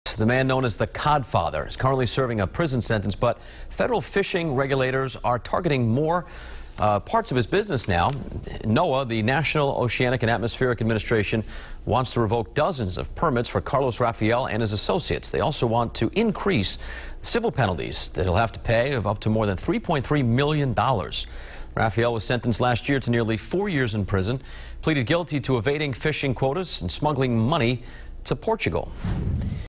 NOAA wants more penalty for 'Codfather'
A NEW BEDFORD FISHING MAGNATE -- THE MAN KNOWN AS THE "CODFATHER" -- IS CURRENTLY SERVING A PRISON SENTENCE